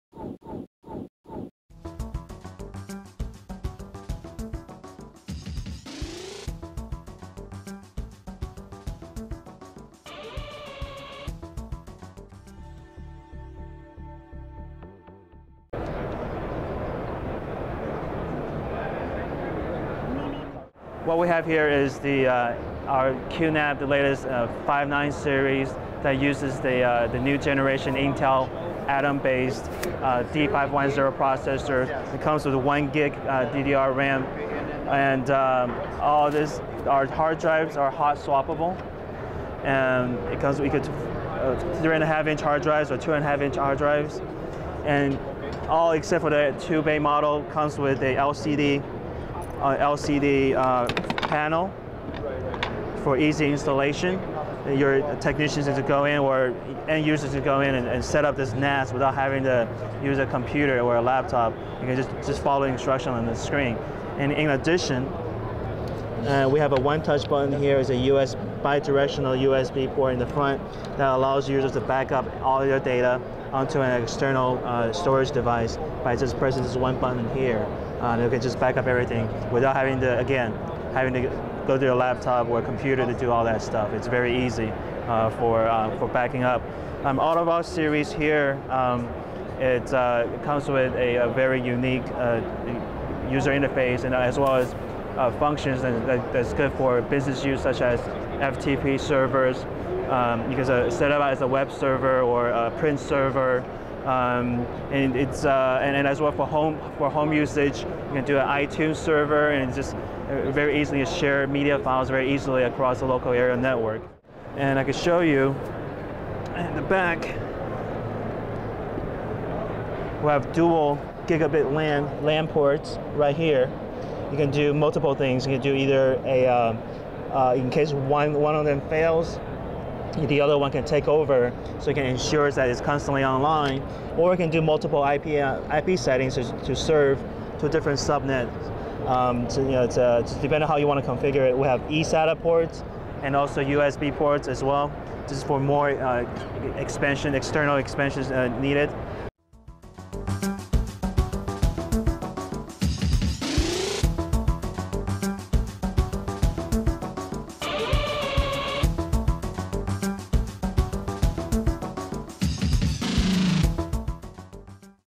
QNAP introduces its fancy new Pinetrail based NAS systems
CES 2010 We stopped by the folks at QNAP who make its bread and butter by producing a solid range of Network Attached Storage or NAS devices that offer a full range of features for users looking at adding more capacity or backup options to their network. In the video below we get a good introduction of the new QNAP 59 series of products and the new features that they bring to the market.  Distributed by Tubemogul.